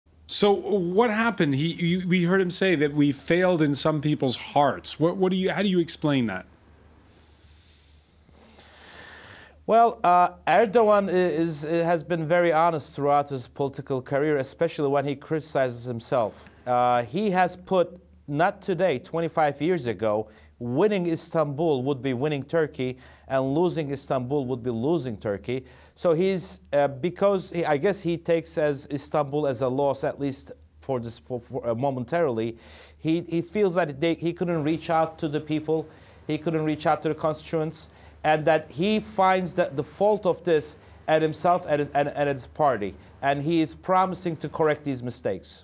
What went wrong for Erdogan in Istanbul?
Onur Erim, former chief adviser to Ankara municipality, on what caused Erdogan's party the AKP to lose votes in Istanbul